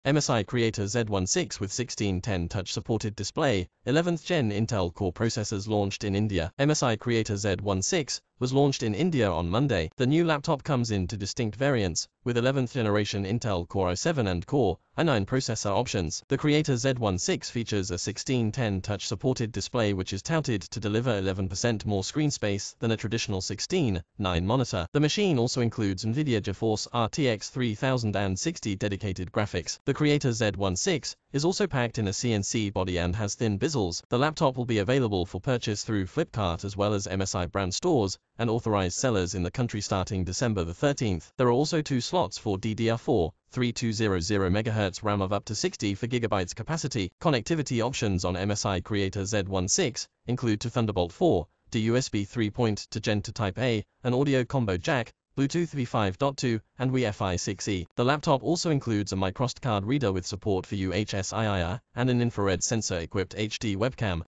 MSI Creator Z16 With 16:10 Touch-Supported Display, 11th-Gen Intel Core Processors Launched in India
MSI Creator Z16 was launched in India on Monday. The new laptop comes in two distinct variants — with 11th-generation Intel Core i7 and Core i9 processor options. The Creator Z16 features a 16:10 touch-supported display which is touted to deliver 11 percent more screen space than a traditional 16:9 monitor. The machine also includes Nvidia GeForce RTX 3060 dedicated graphics. Other highlights of the MSI laptop for creators and professionals include a mini-LED backlit keyboard, Wi-Fi 6E connectivity, and 180W charging. The Creator Z16 is also packed in a CNC body and has thin bezels.   MSI Creator Z16 price in India, availability    MSI Creator Z16  price in India has been set at Rs. 2,40,990 for the Intel Core i7 model, while the Intel Core i9 variant is priced at Rs. 2,57,990. The laptop will be  available for purchase  through  Flipkart  as well as  MSI  brand stores and authorised sellers in the country starting December 13.   MSI Creator Z16 specifications   The new MSI Creator Z16 comes with a 16-inch QHD+ (2,560x1,600 pixels) touch display with a 16:10 aspect ratio. The display also has a refresh rate of 120Hz and 100 percent DCI-P3 colour gamut coverage. Under the hood, the laptop has up to Intel Core i9 processor, along with Nvidia GeForce RTX 3060 GPU with dedicated 6GB GDDR6 graphics memory. There are also two slots for DDR4-3200MHz RAM of up to 64GB capacity. Further, the laptop has two NVMe M.2 SSD storage slots.          MSI has offered the mini-LED backlit keyboard on the Creator Z16 which is claimed to illuminate lightly dim spaces. The laptop also has personalised per-key RGB backlight.   For content creators and designers, the Creator Z16 has factory-calibrated Delta-E  2 colour accuracy out-of-the-box and True Color Technology post the colour gamut.   Connectivity options on MSI Creator Z16 include two Thunderbolt 4, two USB 3.2 Gen 2 Type-A, an Audio combo jack, Bluetooth v5.2, and Wi-Fi 6E. The laptop also includes a microSD card reader with support for UHS-III and an Infrared (IR) sensor-equipped HD webcam.            MSI Creator Z16 packs a four-cell lithium-polymer battery which is paired with a 180W slim adapter. The laptop measures 359x256x16.18mm and weighs 2.2kg. Lastly, it runs on  Windows 10  and is upgradable to  Windows 11 .